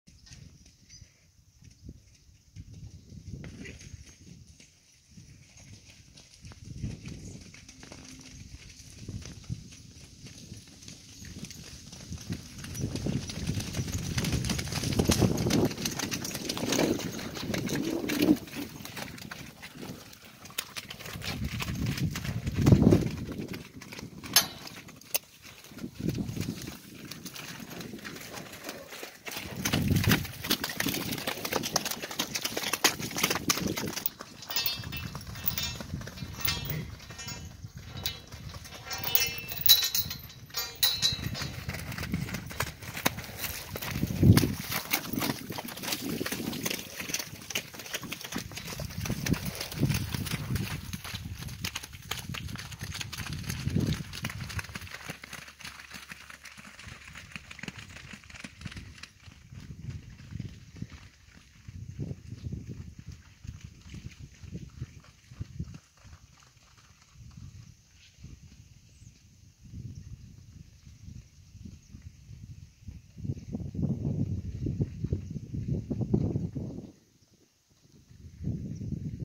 CHASE HER! Horses Meet!
Bogi the Blue Roan mare is introduced to our horses. She is quite polite and keeps a few steps ahead of everyone nicely. Beautiful horses! Farmer Always!